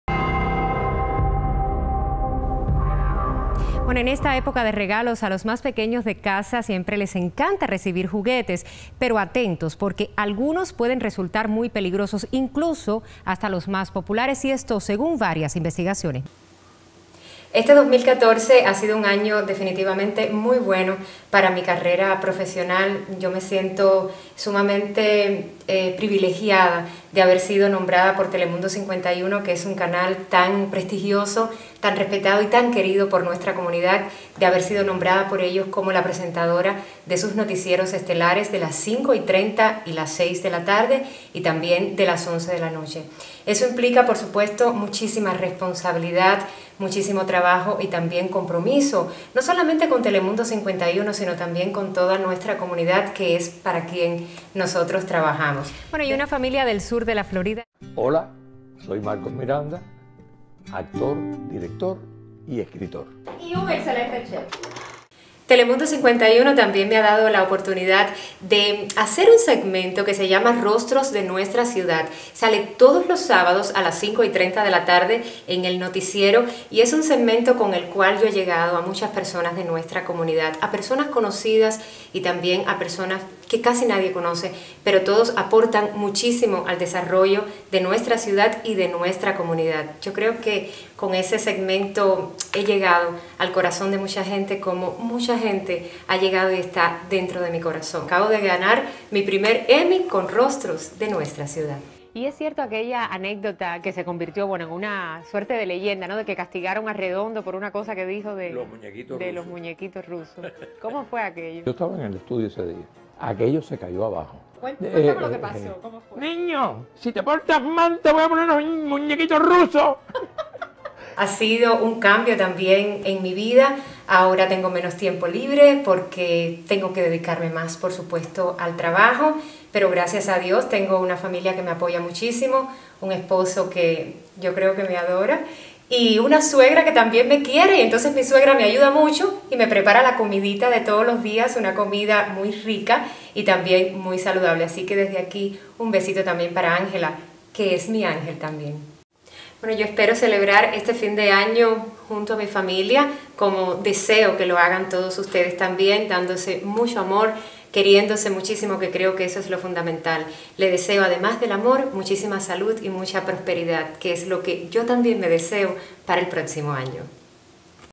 Daisy Ballmajó: "Me siento orgullosa y privilegiada..."
Daisy Ballmajó ya lleva más de un año como el rostro de las noticias en Telemundo 51, el espacio local de la cadena Telemundo, en los horarios de mayor audiencia, a las 5:30, las 6 y las 11 p.m.

“Ha sido un año de gran crecimiento”, reconoce la presentadora cubana. “Me siento muy orgullosa y privilegiada de que Telemundo 51, un canal tan relevante y querido por nuestra comunidad, me haya nombrado como presentadora de sus noticieros estelares”.
(Video: Pedro Portal)